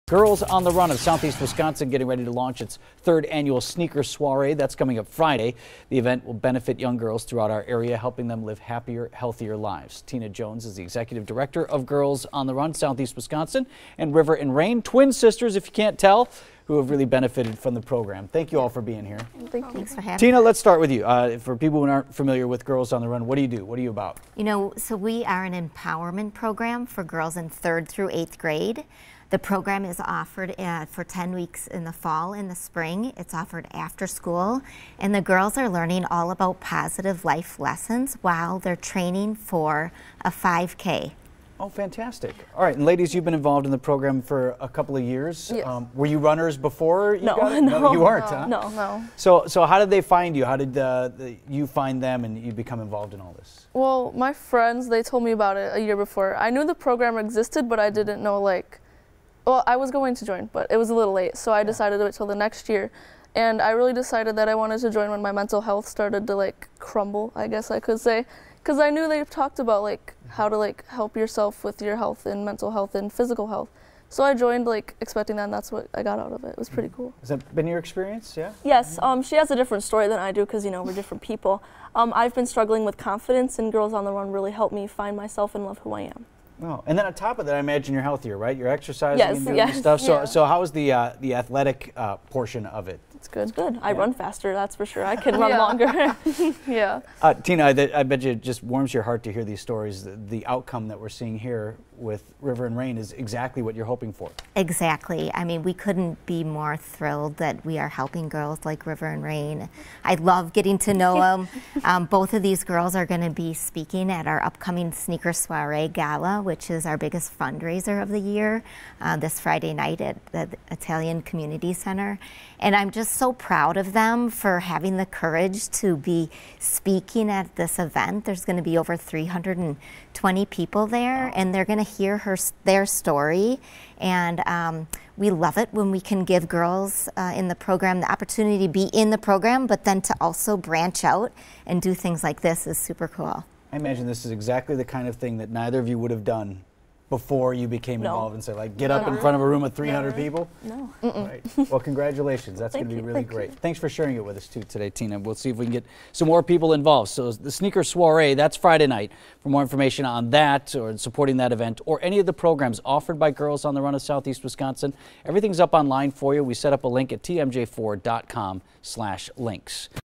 Girls on the run fundraiser
Girls on the Run is an empowerment program for girls training for a 5K.